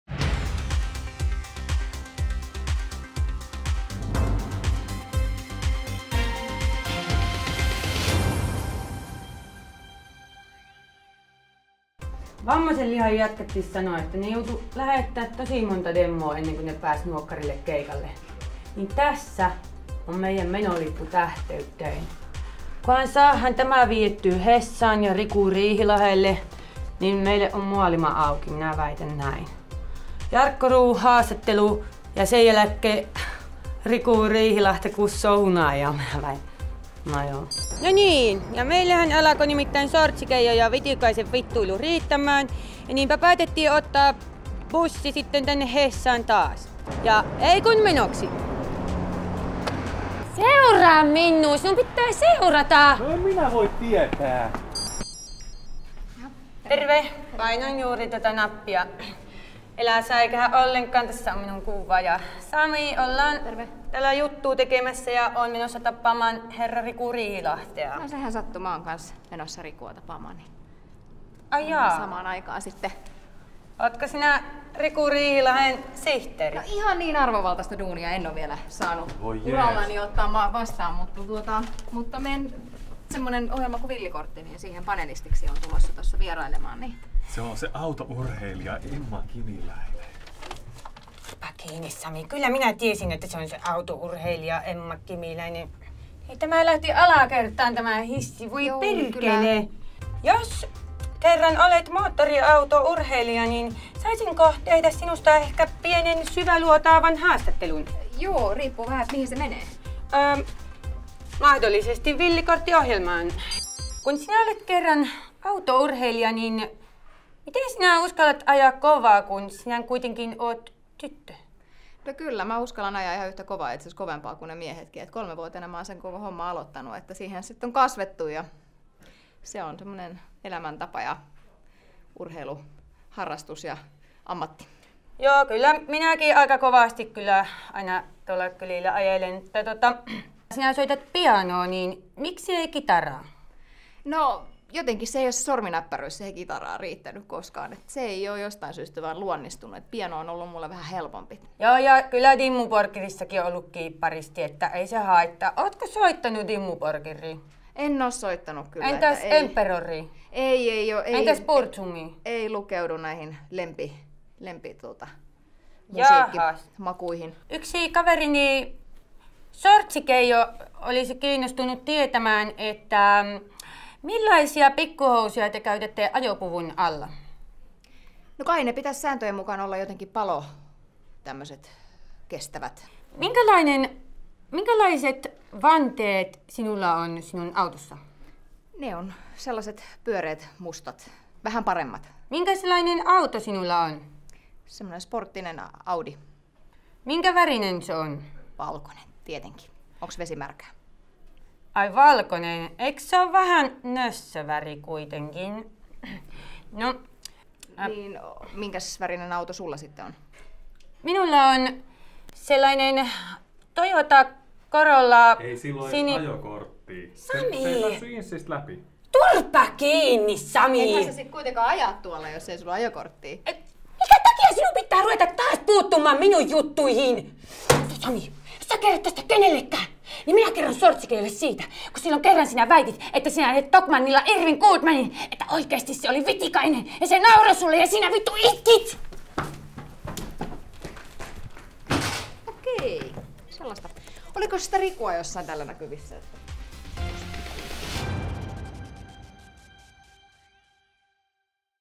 Syväluotava haastattelu Emma Kimiläinen autourheilija (ei räikkönen)
Oltii Samin kansa vähä haastattelun myyntihommissa Hesassa ja Haastateltii autourheilija Emma Kimiläistä.